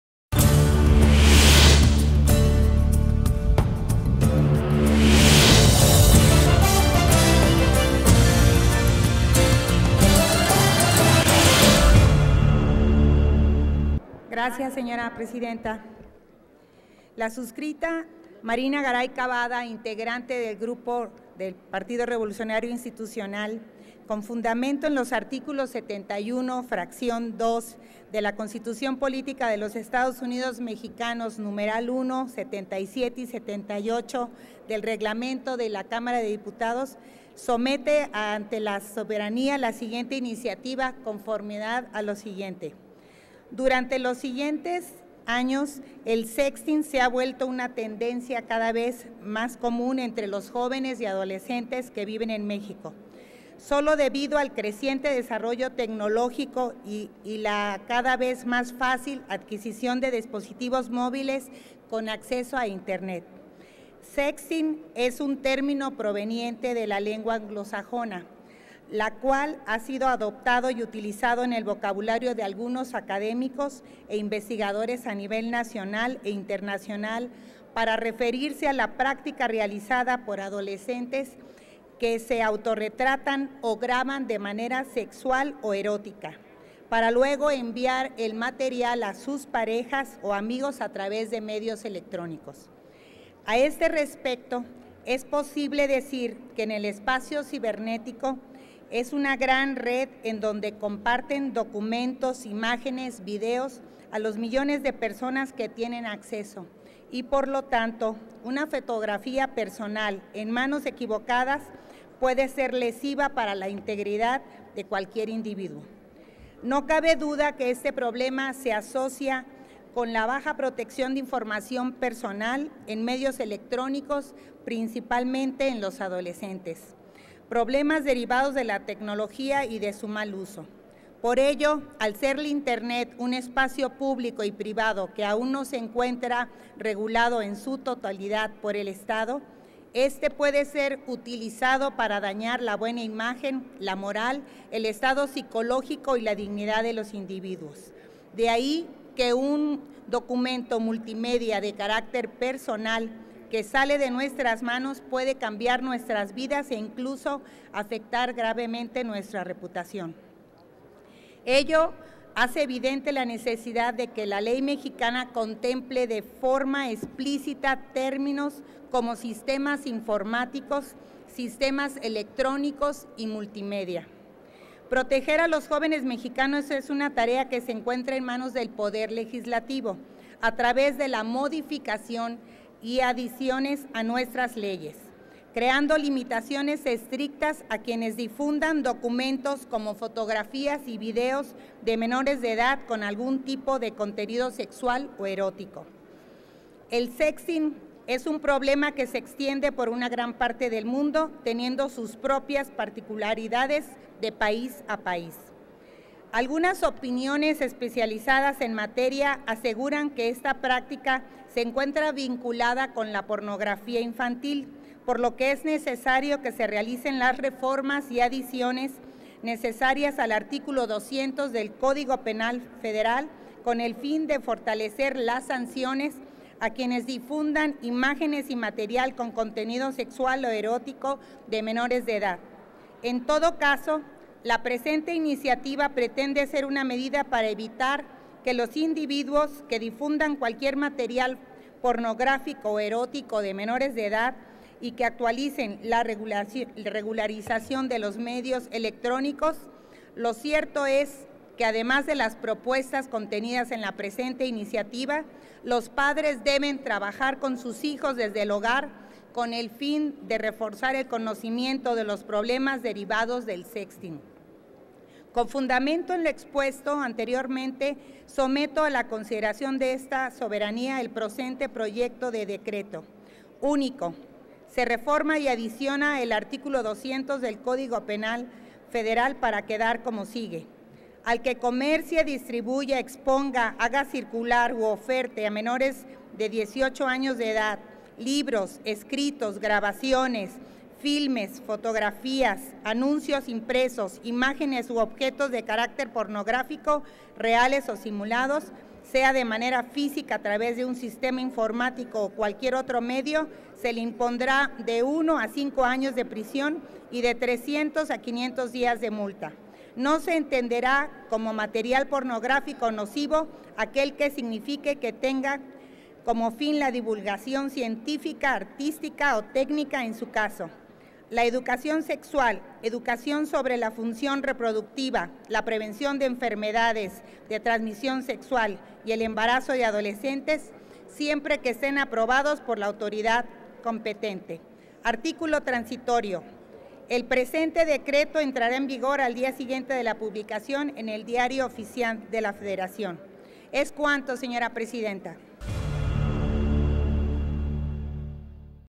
Dip. Marina Garay Cabada (PRI) - Art. 200 del Código Penal Federal.
La iniciativa de la Dip. Marina Garay Cabada (PRI) tiene por objeto sancionar al que comercie, distribuya, exponga, haga circular u oferte a menores de 18 años material pornográfico a través de un sistema informático.